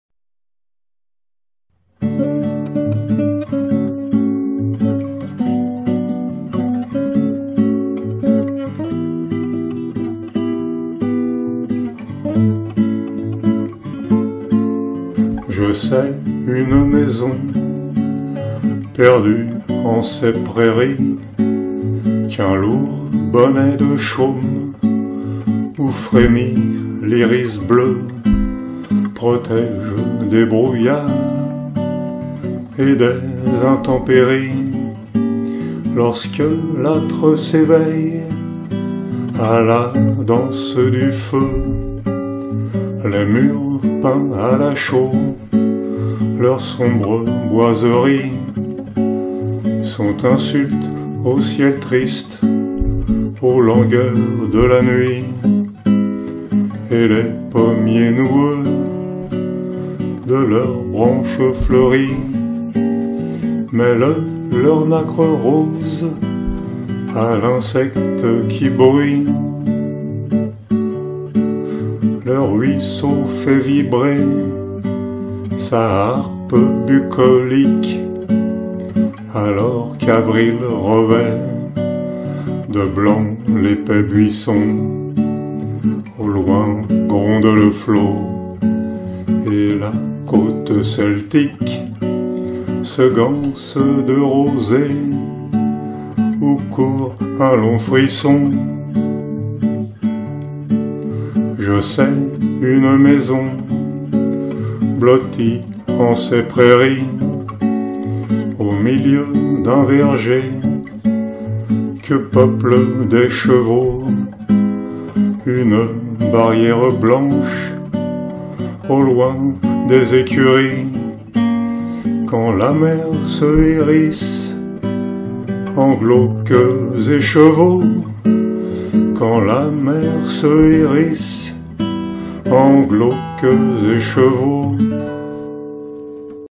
Sous les ailes des mouettes (Suzanne Gourin)
VIDÉO À BUT NON LUCRATIF ET NON COMMERCIAL
Suzanne Gourin est une poétesse française née à Limoges le 18 décembre 1930 et décédée le 15 février 2018. Elle laisse derrière elle une œuvre achevée et un palmarès de distinctions impressionnant.

Under the wings of seagulls (Suzanne Gourin)
I know a house lost in these meadows,
That a heavy thatched hat where the blue iris quivers,
Protects from fogs and bad weather,
When the hearth awakens to the dance of fire.

The whitewashed walls, their dark woodwork,
Insult the sad sky, the languors of the night,
And the gnarled apple trees, with their flowering branches,
Mix their pink mother-of-pearl with the rustling insect.

The stream vibrates its bucolic harp,
As April whitens the thick bush
In the distance rumbles the flood, and the Celtic coast,
Gathered in dew where a long shiver runs.

I know a house nestled in these meadows,
In the middle of an orchard populated by horses
A white barrier, far from the stables,
When the sea bristles in gloomy skeins.
Sous les ailes des mouettes.
Je sais une maison perdue en ces prairies,
Qu’un lourd bonnet de chaume où frémit l’iris bleu,
Protège des brouillards et des intempéries,
Lorsque l’âtre s’éveille à la danse du feu.

Les murs peints à la chaux, leurs sombres boiseries,
Sont insulte au ciel triste, aux langueurs de la nuit,
Et les pommiers noueux, de leurs branches fleuries,
Mêlent leur nacre rose à l’insecte qui bruît.

Le ruisseau fait vibrer sa harpe bucolique,
Alors qu’avril revêt de blanc l’épais buisson
Au loin gronde le flot, et la côte celtique,
Se ganse de rosée où court un long frisson.

Je sais une maison blottie en ces prairies,
Au milieu d’un verger que peuplent des chevaux
Une barrière blanche, au loin des écuries,
Quand la mer se hérisse en glauques écheveaux.